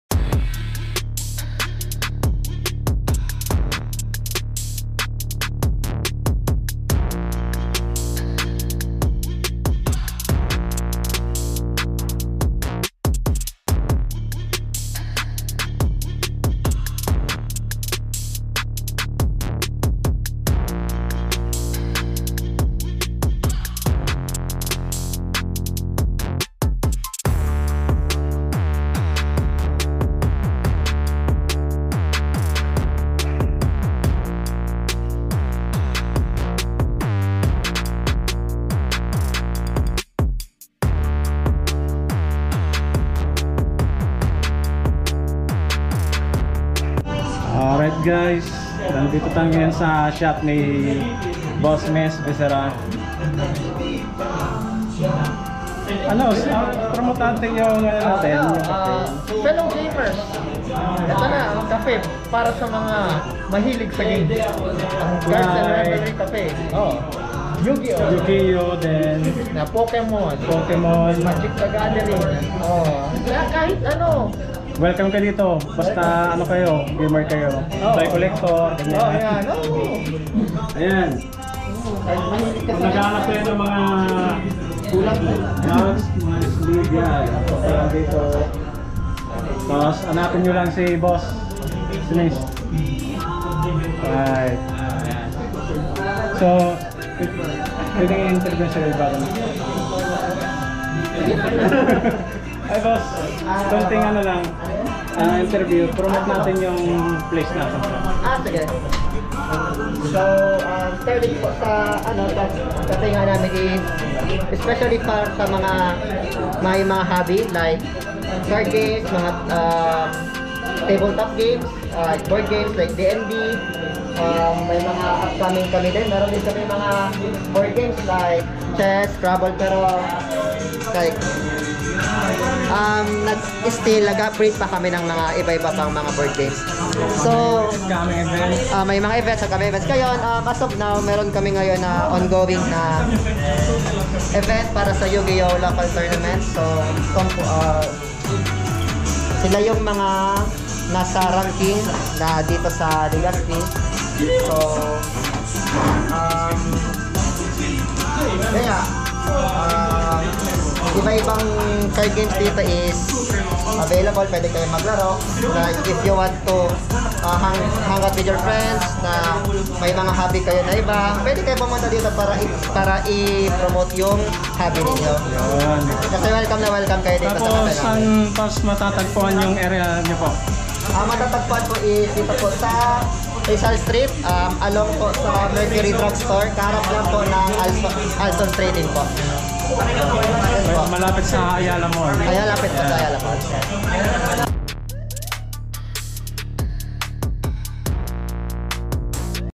Episode 124 - Cards And Revelry Cafe
Sa mga bagong heneration ng mga kabataan na mahihilig sa card games, at isang toy collector, duelist, player, etc, punta lang po sa Cards and revelry Cafe na matatagpuan sa Door 7, Ground Floor, Pineda Bldg. 1, Rizal Street, Legazpi City, Albay 4500, Malapit sa Ayala Mall, harap ng Alson.

Pag pumunta ka dito matatanggal stress mo makakapag relax ka pa with your friends,

meron din silang mga tinitinda like MTG  cards, YUGIOH cards, Pokemon cards and digimon cards, meron din silang card protection sleeve and card collector album.